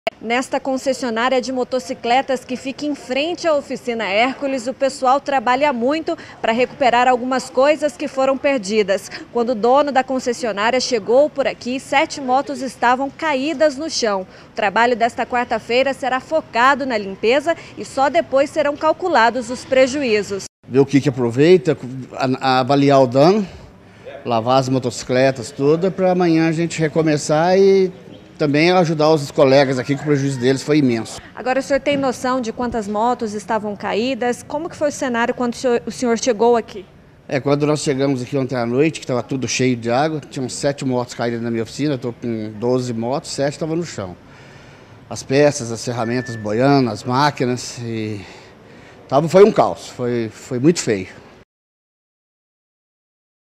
ESTRAGOS CONCESSIONÁRIA MOTOS
Concessionária de motos, que fica na Rua Capitão Afonso Junqueira, foi muito afetada